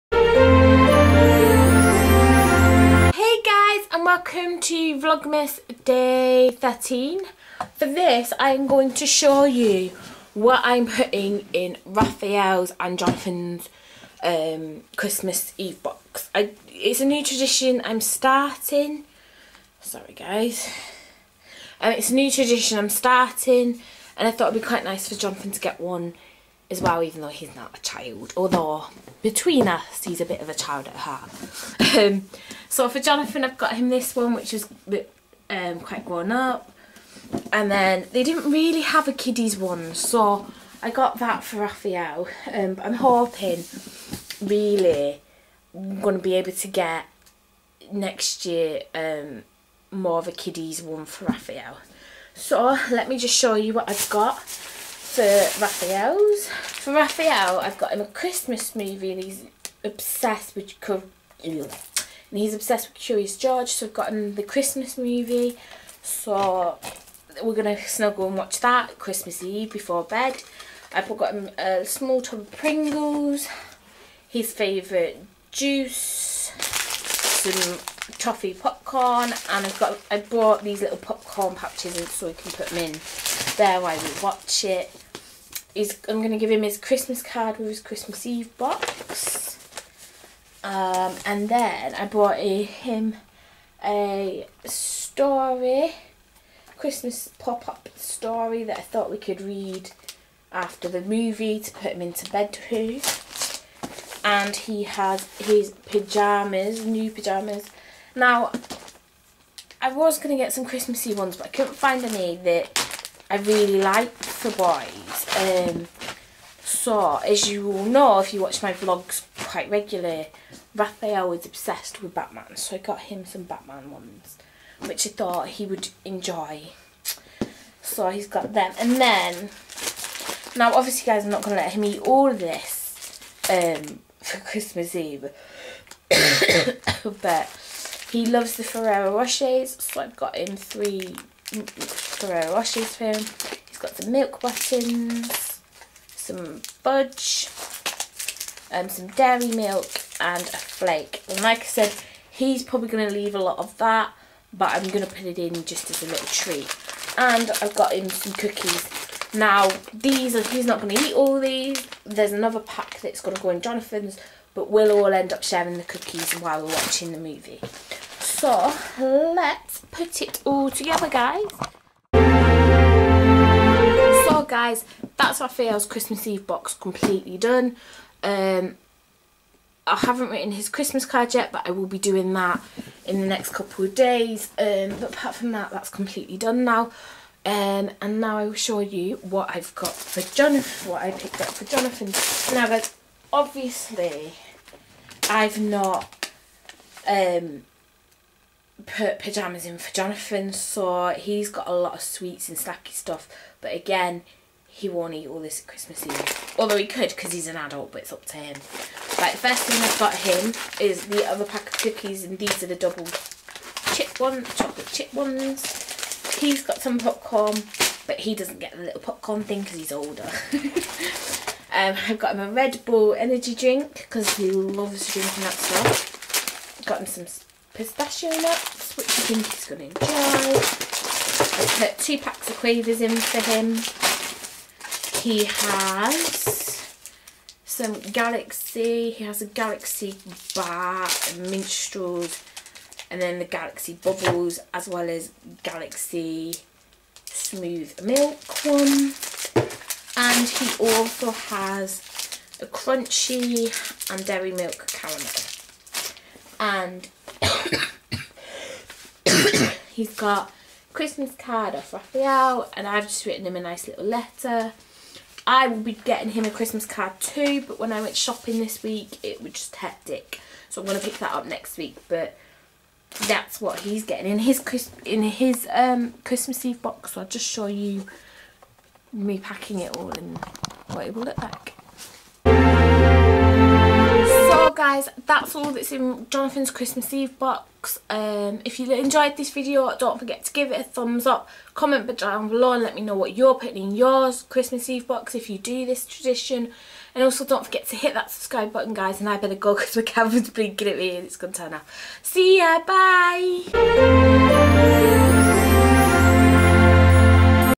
Vlogmas Day 13 - Christmas Eve Boxes
Hey guys as promise here is the second video today I will upload another two tomorrow and I should be straight then :D. Here is Raphael's and Jonathan's christmas eve boxes